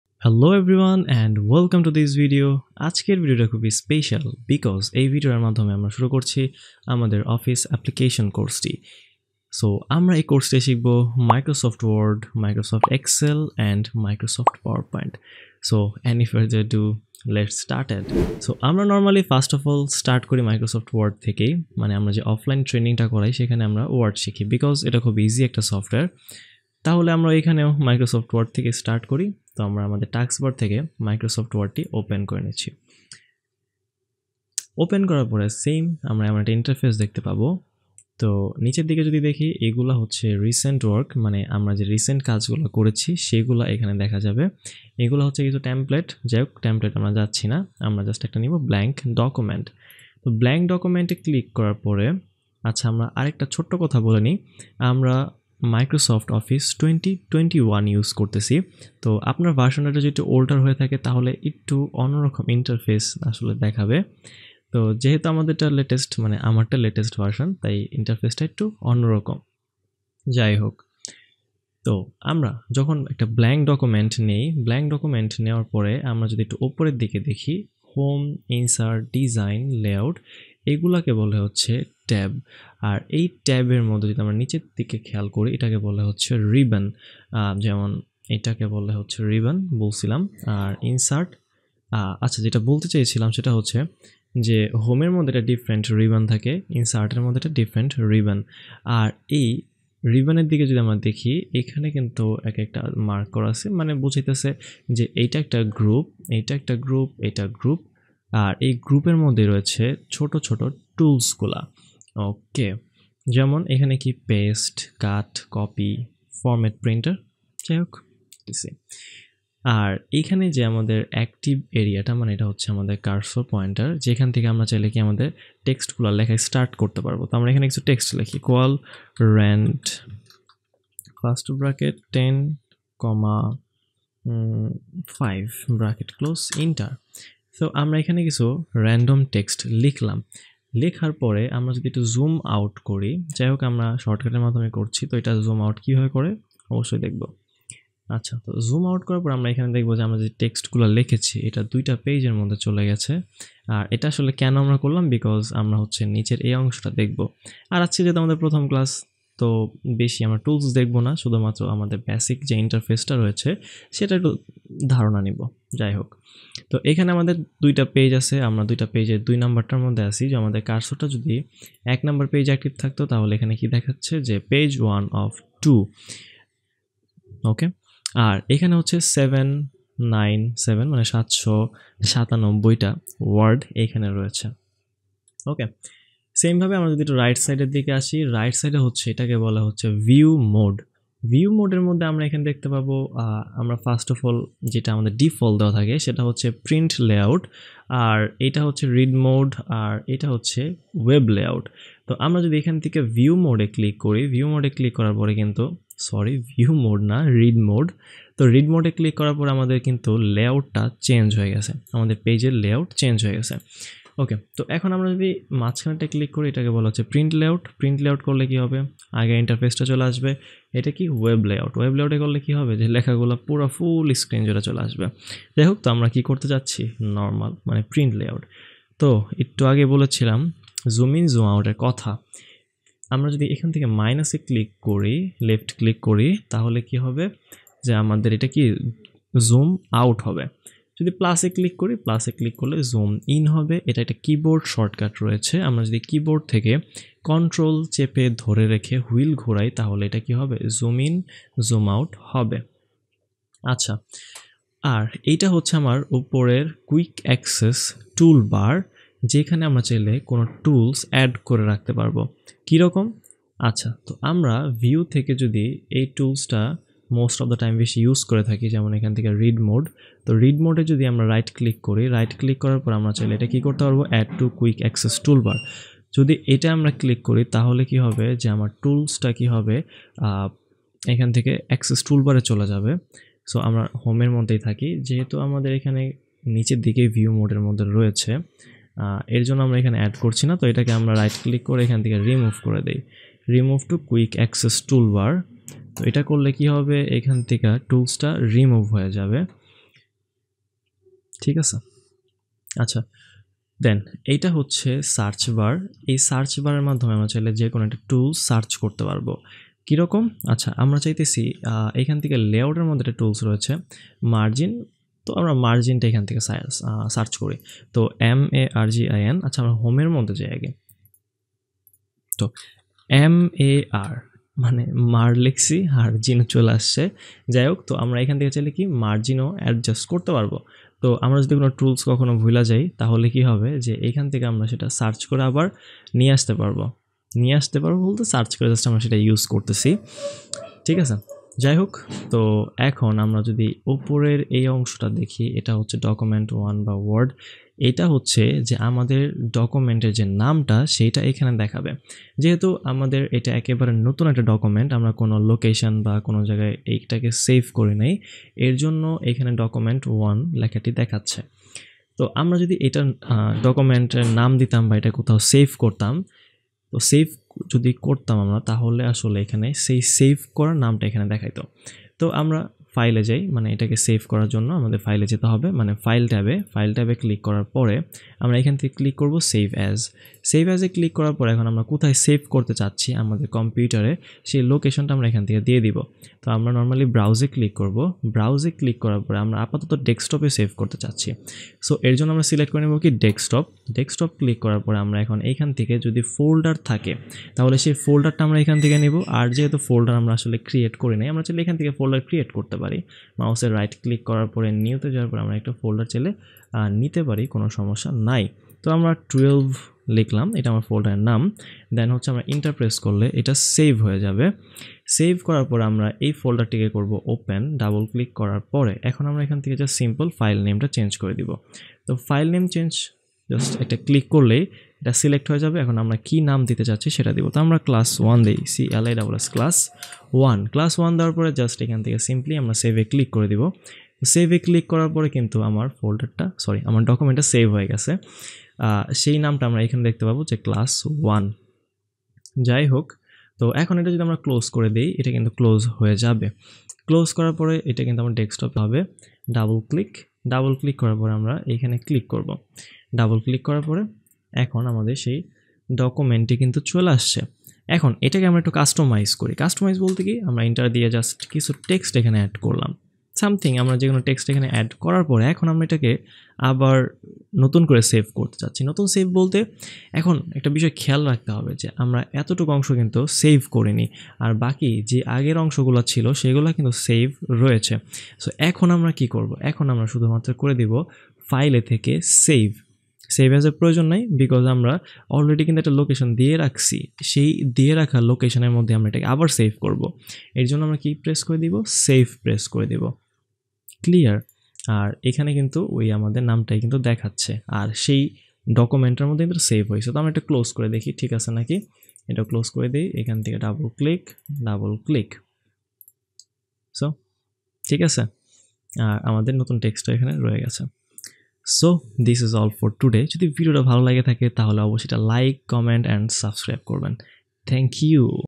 MS Word Full Course for Beginners Class-1 . With Bangla Tutorial
MS Word - Basics
Lecture By: Mridul Ahmed Quick Learning Academy ...
In this video learn Basic Intro, How to Seve a new document in Microsoft word..